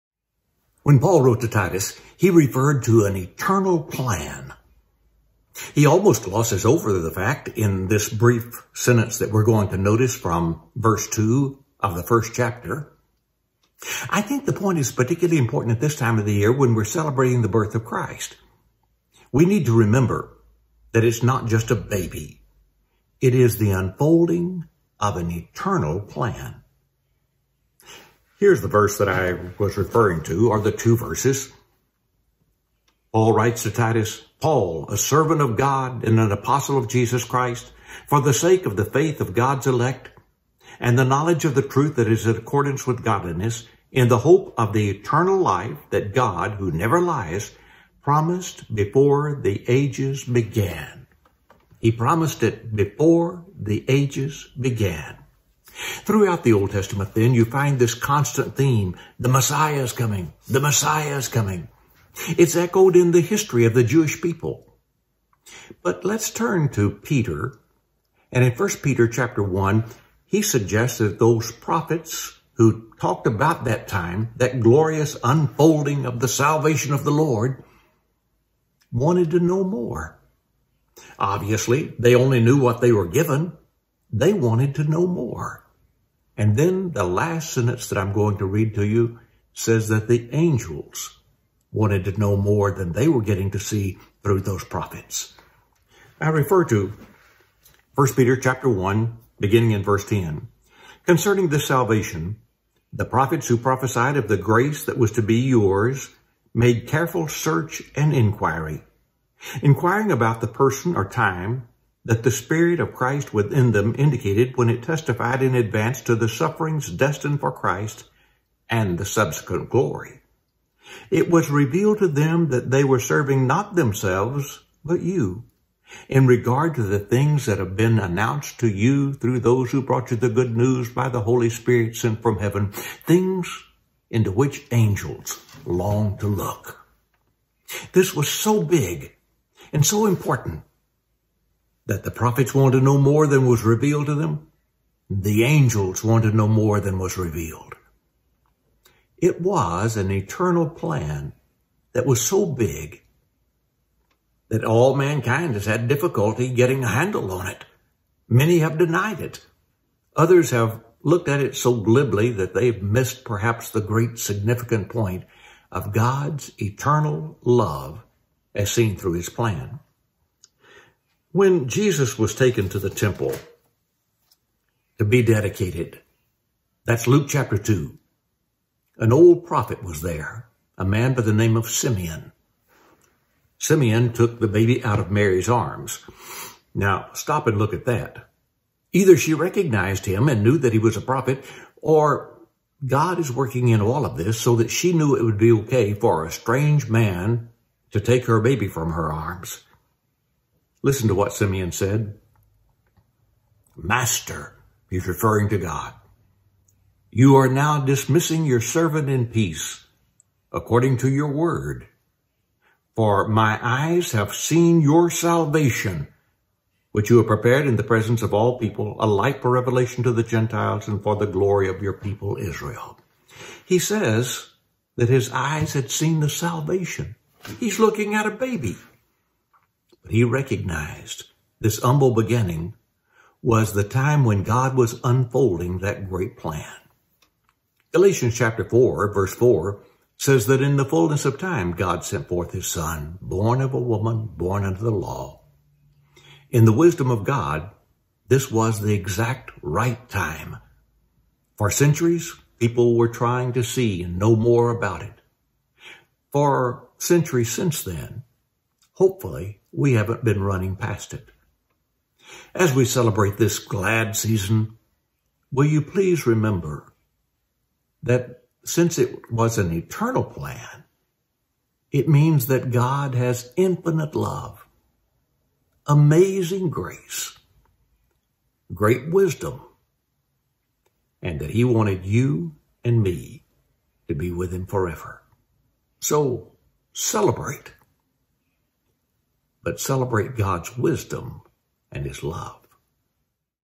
Moments with Jim: Eternal Plan
As we celebrate this season, let’s celebrate the eternal plan for our salvation that was unfolding in the birth of Jesus.